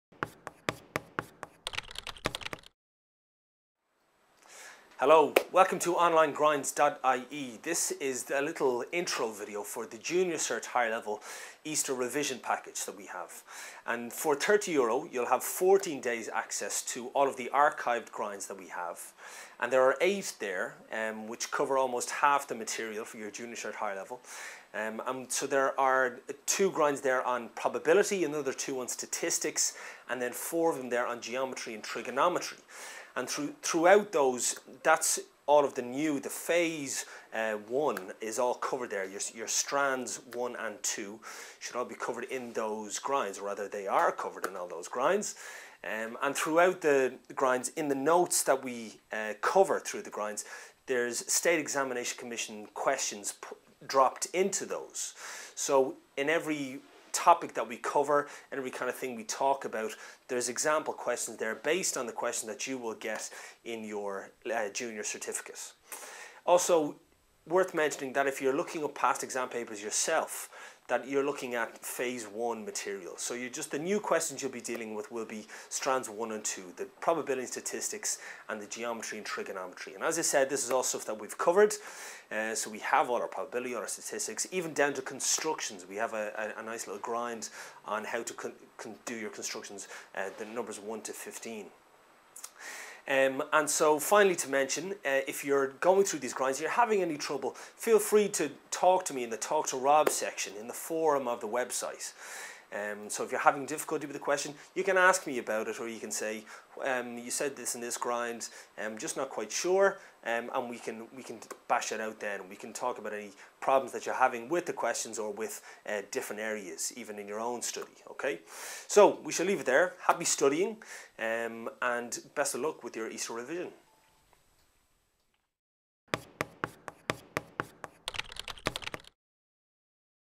OnlineGrinds.ie Junior Cert Higher Level Maths Easter Revision Guide
OnlineGrinds.ie Junior Cert Higher Level Maths Teacher Robert Dempsey gives you advice on how to revise over easter